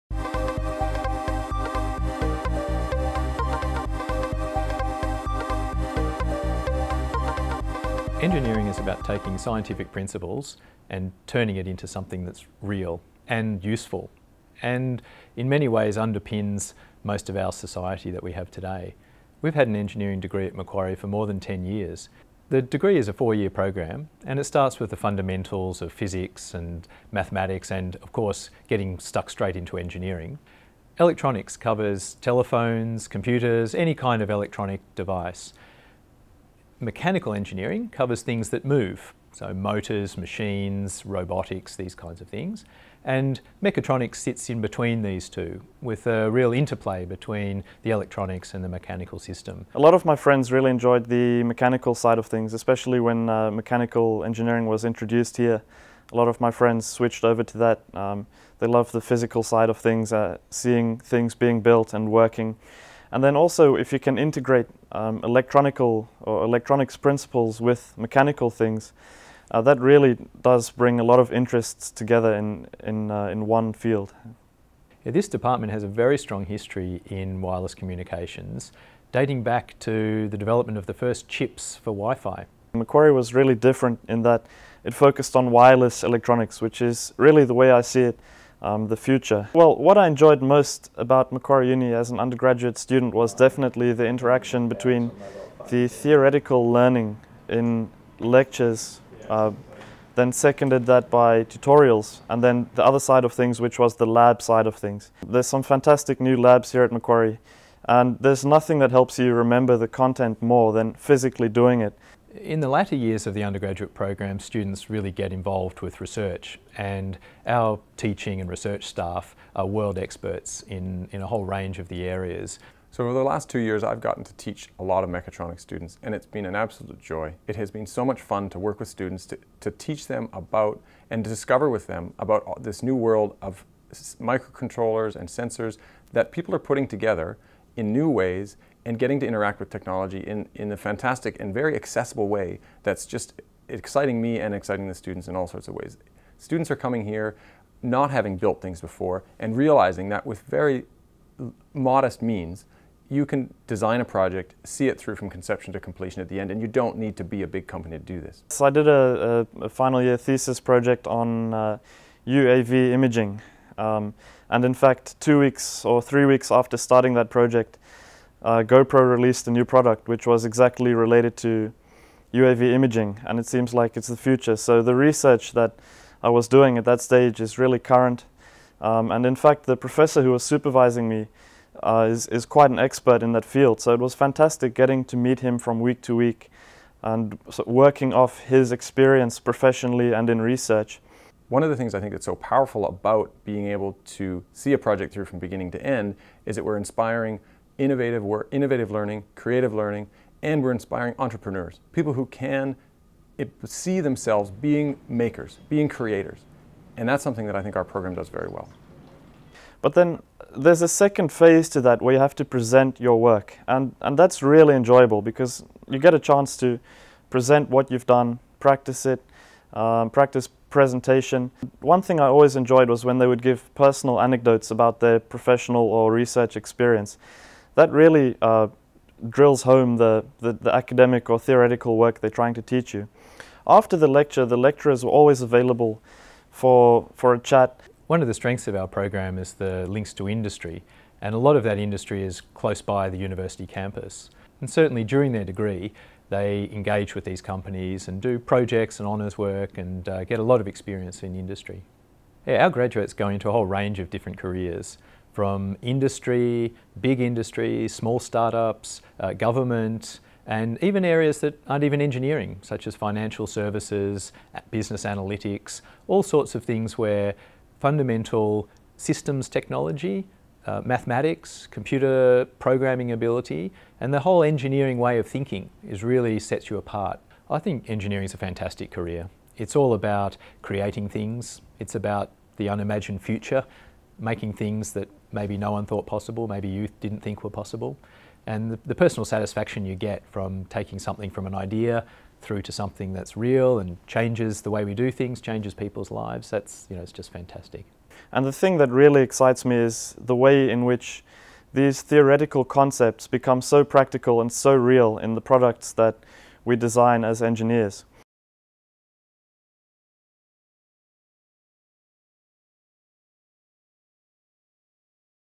Engineering at MQ
What is Engineering? How do we apply it to our everyday life? This video demonstrates how MQ is a world leader in teaching engineering.